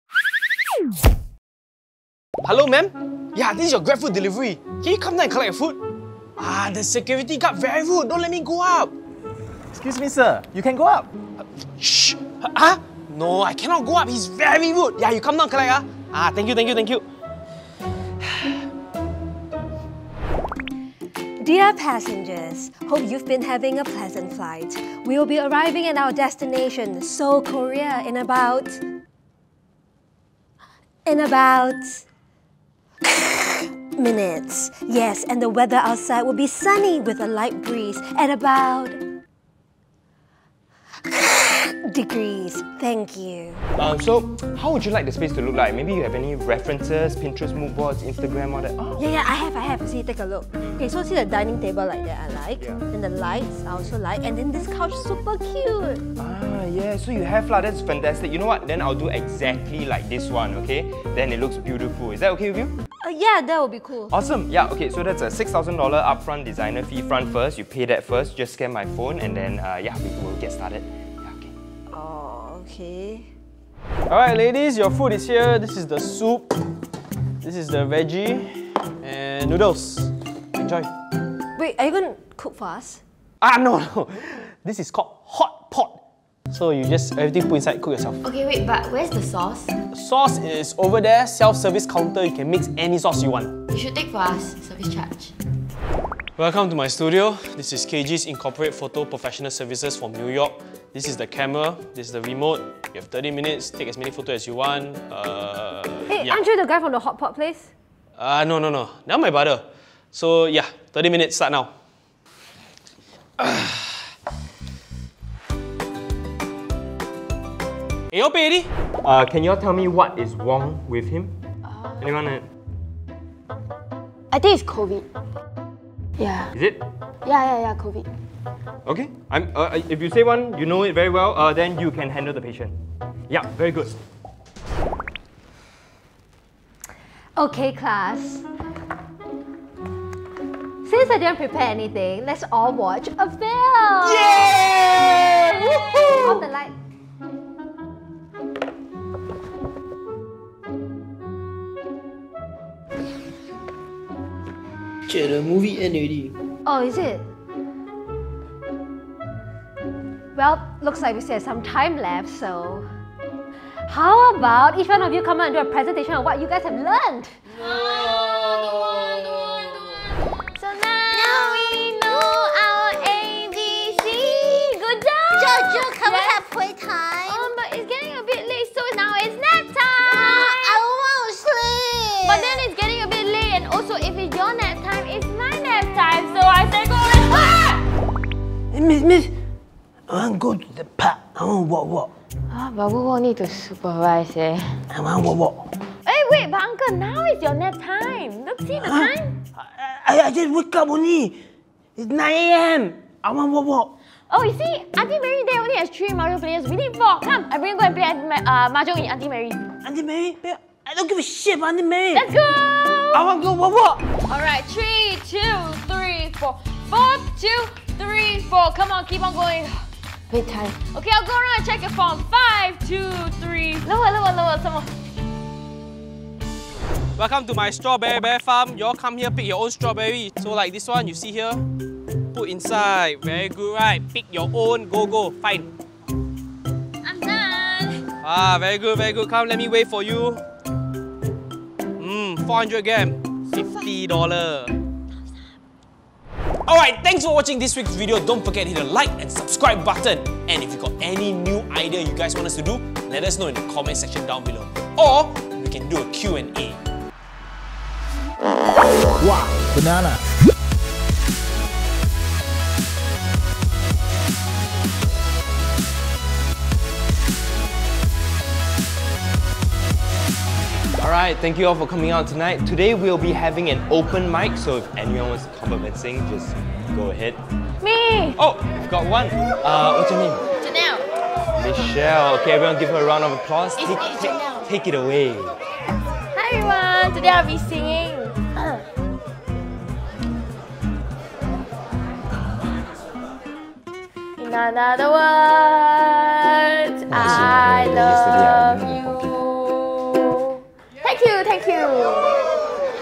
How People Slack At Their Jobs
I mean, if people are still going to pay for it by doing it themselves, then why not hor?

Produced by:
Jeanette Yeap @Jeanettoes
Sefrina Oh @sefrinaoh

Starring:
Keiji Umehara @umeandhara
Kelly Wong @kellykanez
Thomas K @heythomask
Jeynelle Ng @buffbaby88